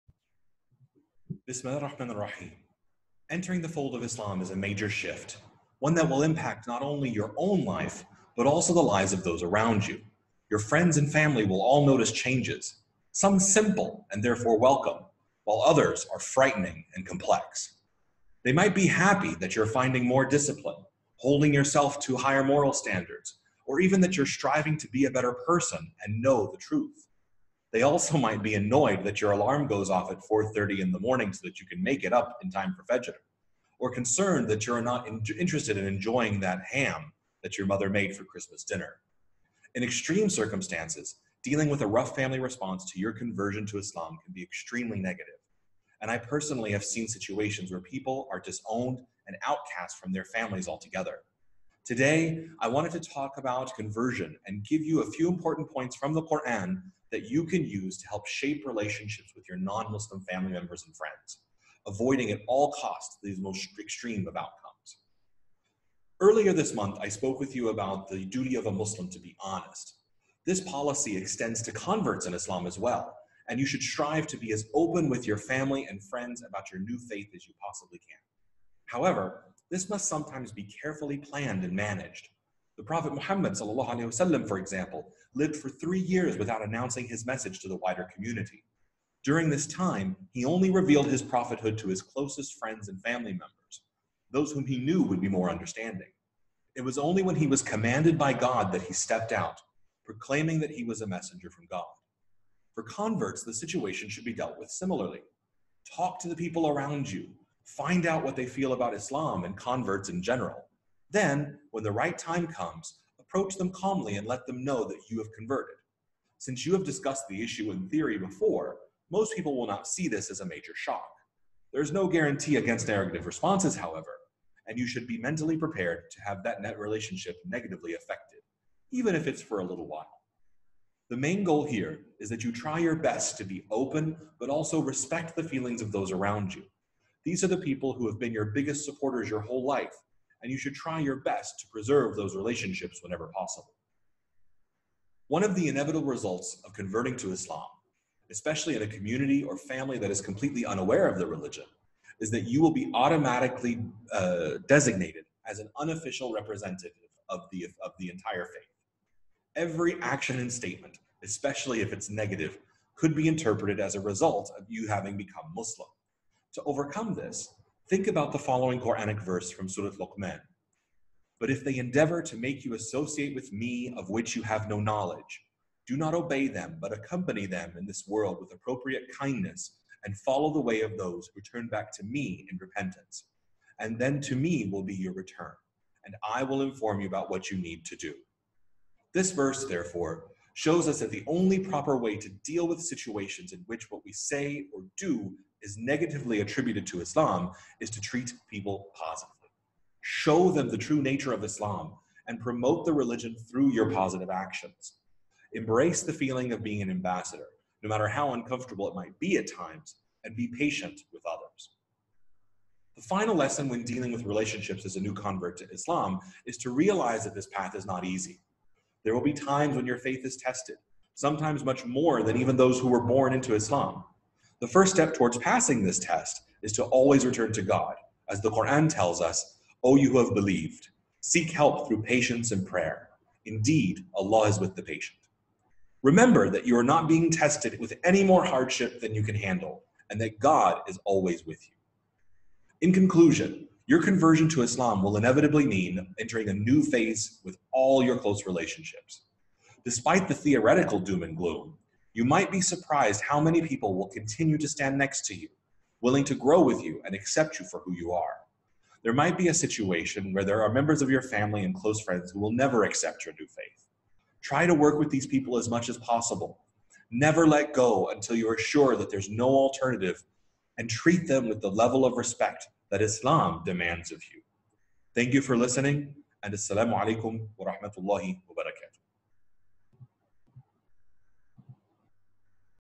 Coping with Conversion and Family Stress
Dealing with a rough family response to a conversion to Islam can be extremely negative. Brian Wright talks about 3 important points to successfully deal with non-Muslim family members and friends.